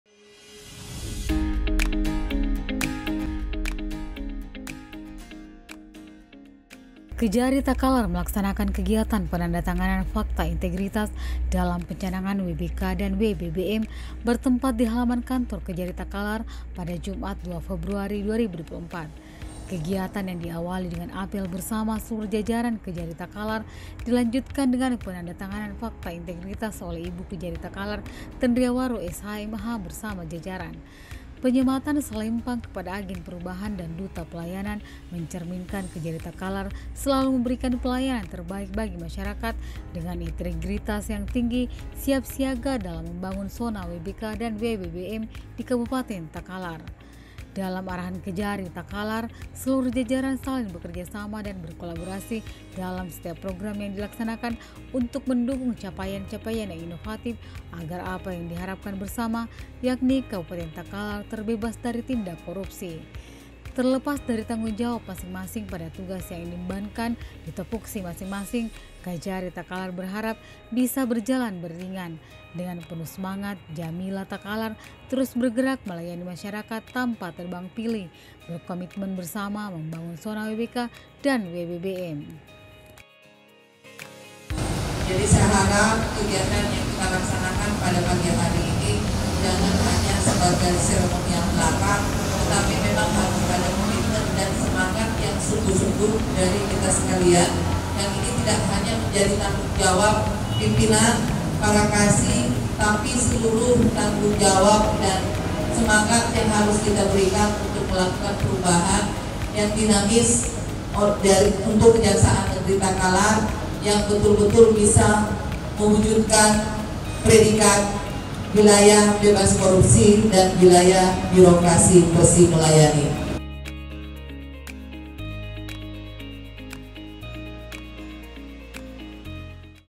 Kejari Takalar Laksanakan Penandatanganan Pakta Integritas dalam rangka Pencanangan WBK & WBBM
Kejasaan Negeri Takalar Laksanakan Penandatanganan Pakta Integritas dalam rangka Pencanangan WBK & WBBM di Halaman Kantor Kejari Takalar pada Jumat 2 Februari 2024